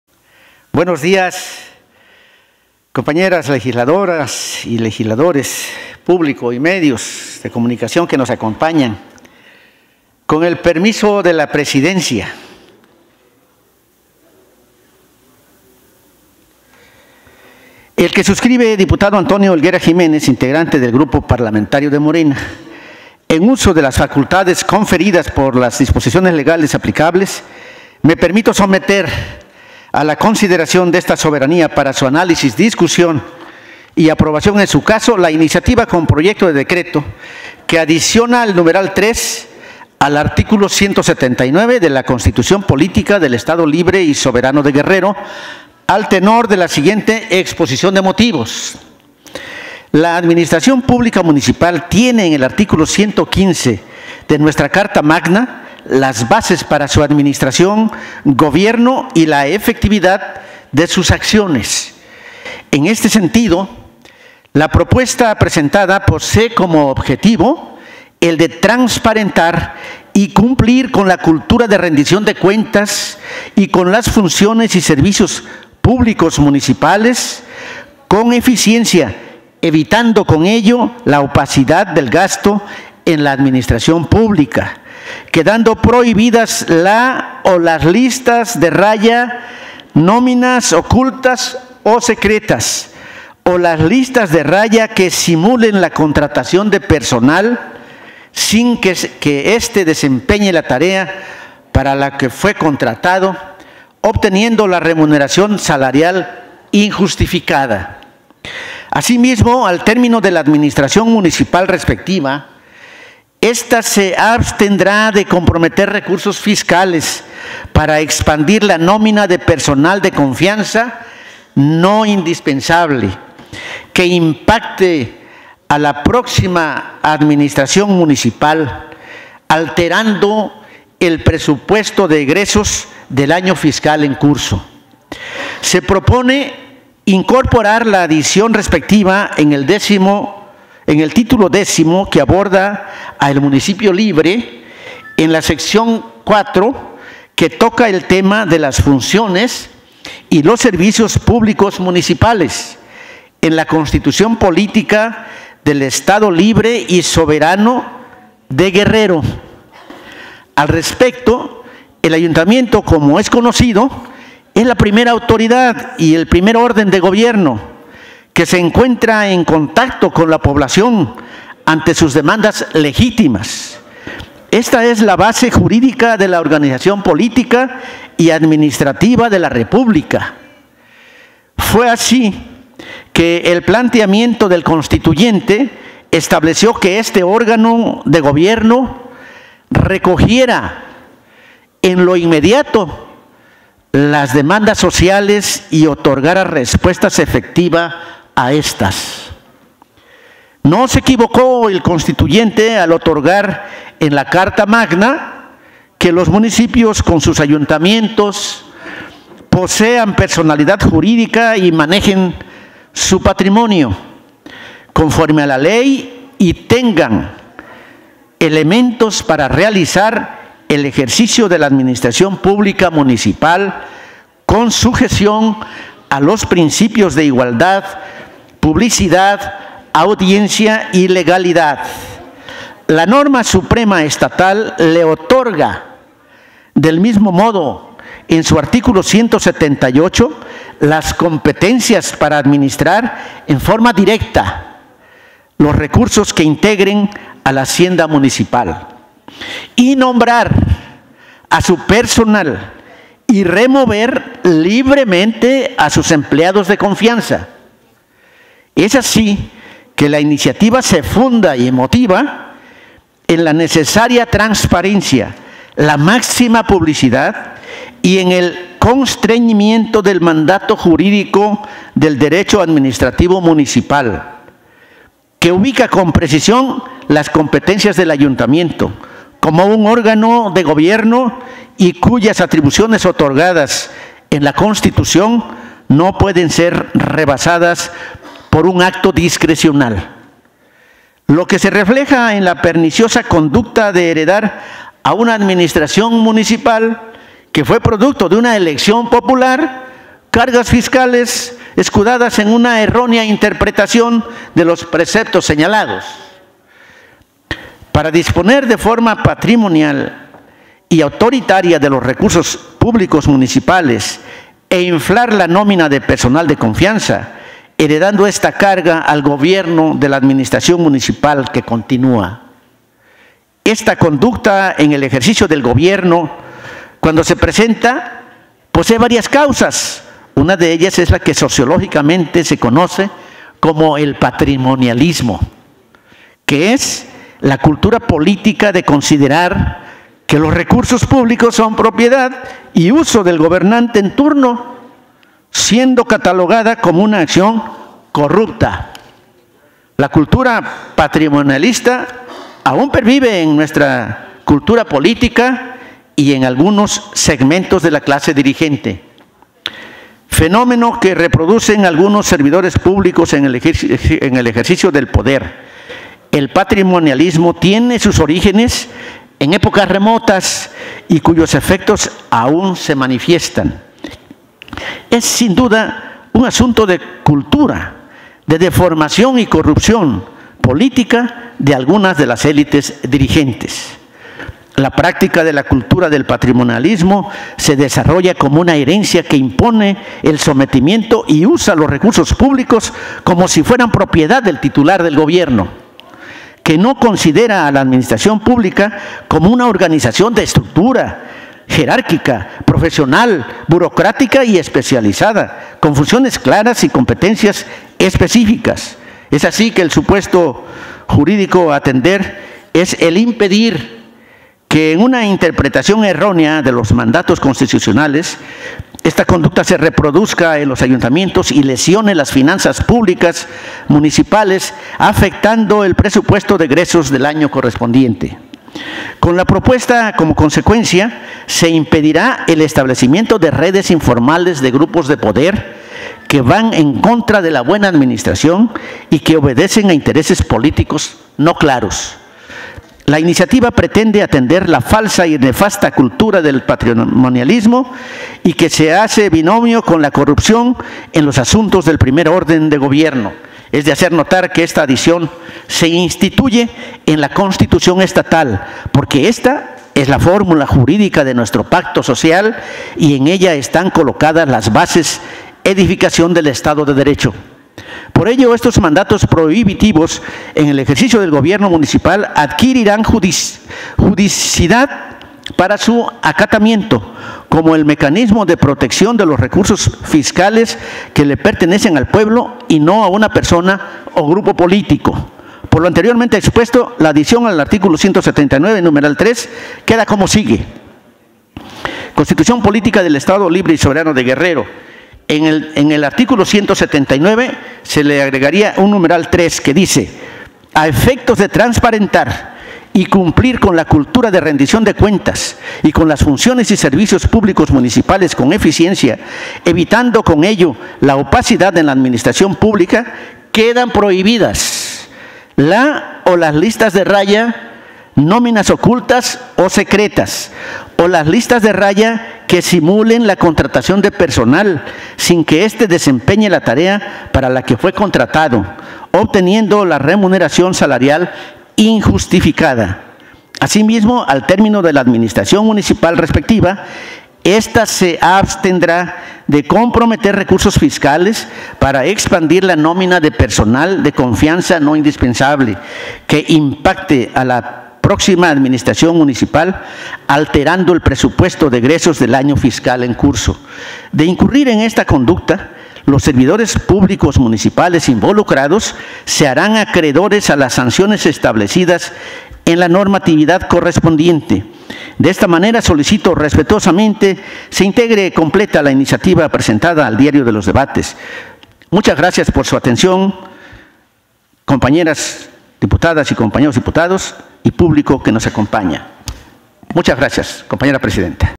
Morena | Iniciativa del Dip. Antonio Helguera Jiménez | 06-06-23
El diputado Antonio Helguera Jiménez (Morena) presentó una iniciativa de adición a la Constitución Política del Estado de Guerrero para establecer la prohibición de la "lista de raya" o “nómina secreta”, que inhiba la opacidad en el funcionamiento de la administración pública municipal y, en caso de incumplimiento, se apliquen las debidas sanciones, de acuerdo con la normatividad vigente. La propuesta se turnó a la Comisión de Estudios Constitucionales y Jurídicos para su correspondiente análisis y dictaminación.